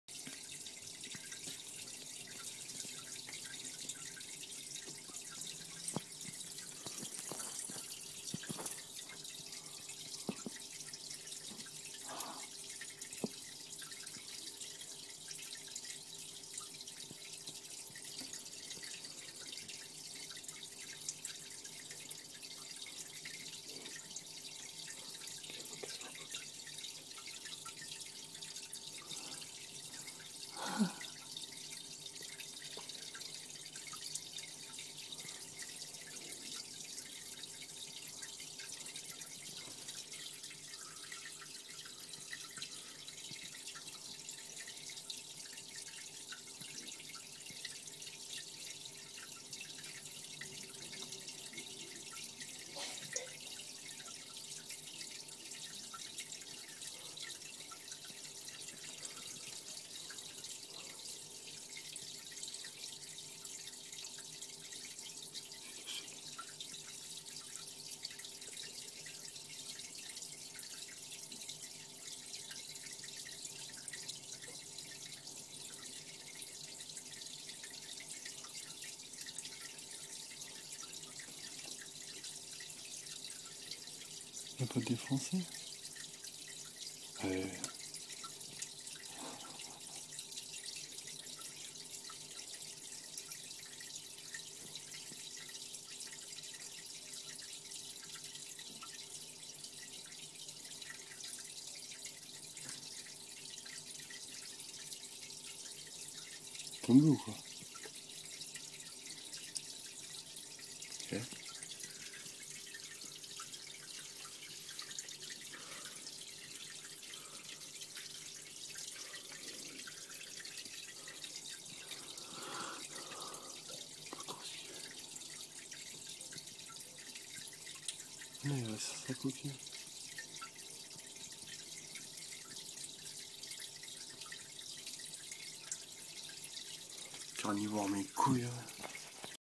Escargot helena
En voulant manger l'escargot le petit la rendu fou.